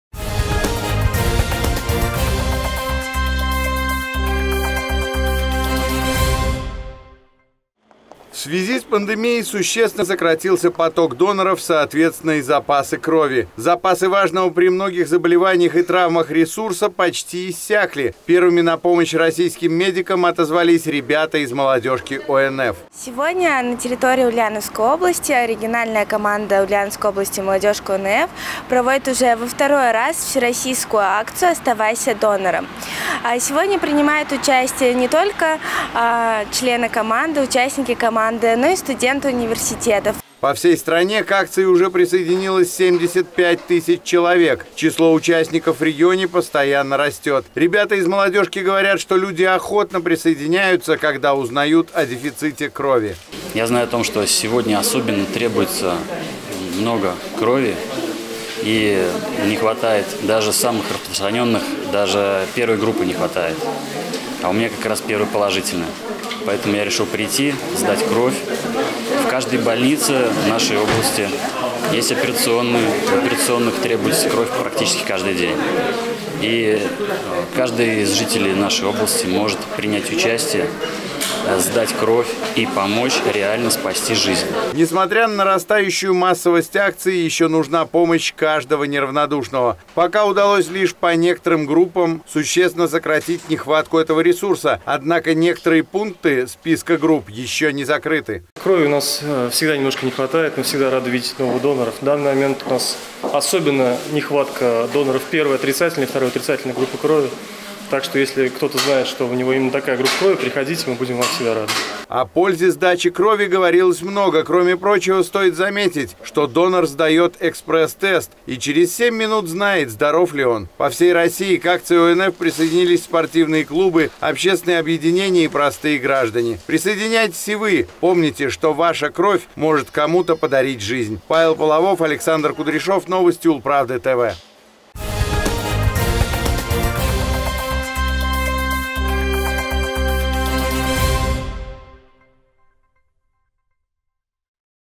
Ульяновская молодежь пополнила ряды доноров
В связи с пандемией существенно сократился поток доноров, соответственно и запасы крови. На помощь российским медикам отозвались ребята из «Молодежки ОНФ».
По всей стране к акции уже присоединилось 75 тысяч человек, число участников в регионе постоянно растет. Ребята из «Молодежки» говорят, что люди охотно присоединяются, когда узнают о дефиците крови.